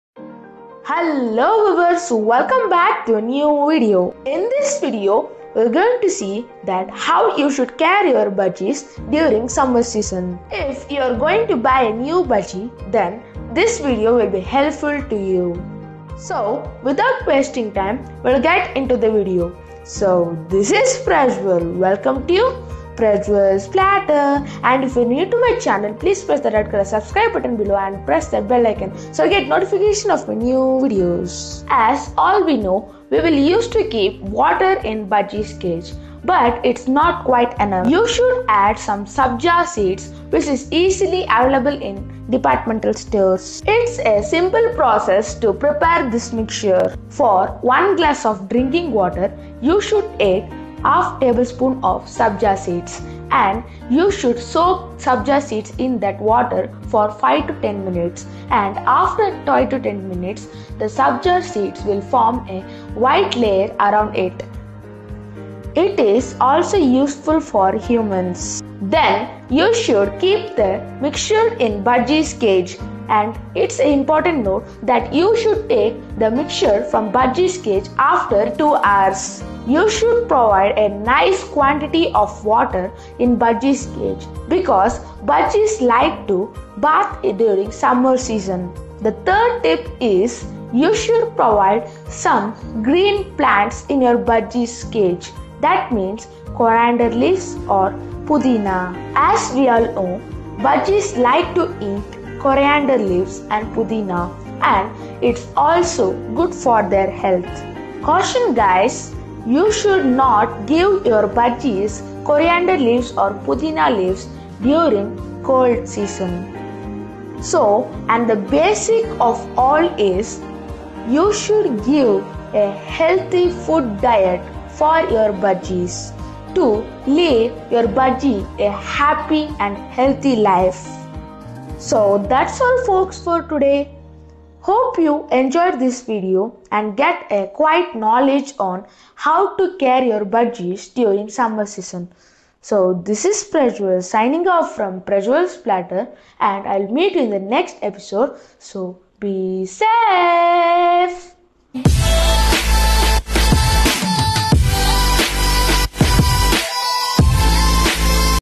How to care budgies in summer season/english||prajwal's platter
hi guys ,

                in this video we are going to see that how we should to care budgies during summer season   . summer season is so hot as we all know . if even we cant maintain the heat then how the birds will.  so there are tips in this video. for how u should care.  so this video is ver very helpful for beginners

every video is made by more and more efforts

pls wathc the video without skipping

video credits:Getty images